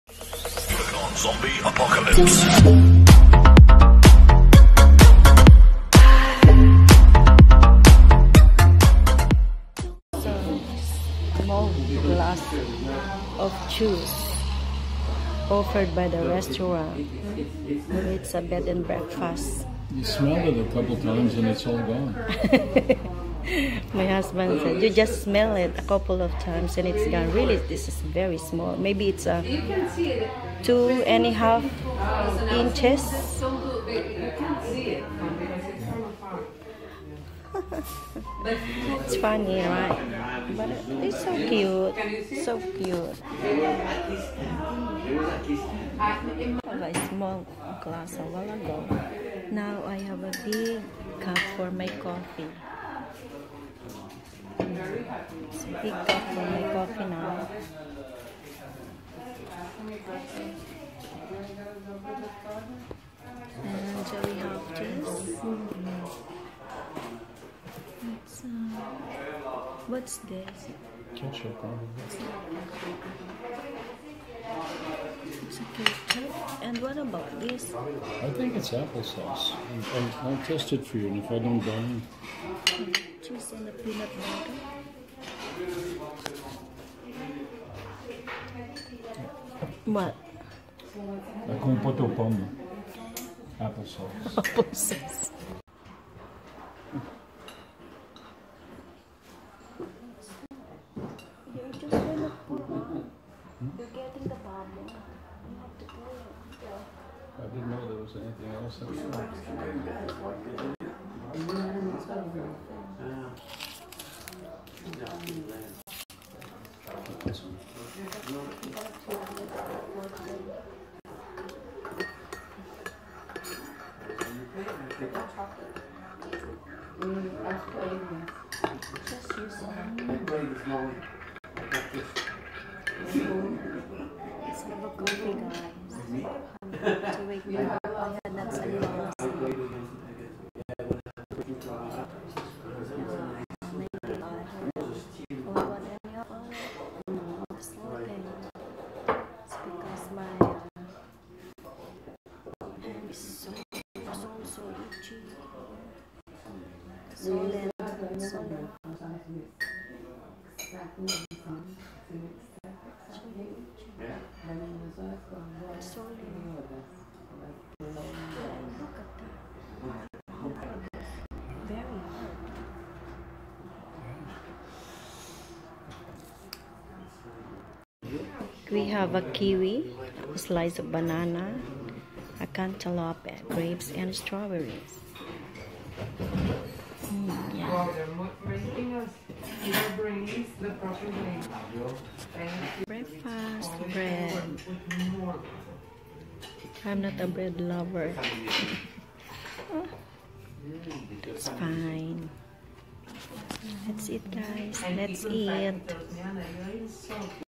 Breakfast at Chez Sam Cafe
We stop at Saint Catherine, Quebec for a night and stayed at Chez Sam Cafe.  We had a simple but good breakfast.  The room is small which is just perfect for an overnight stay.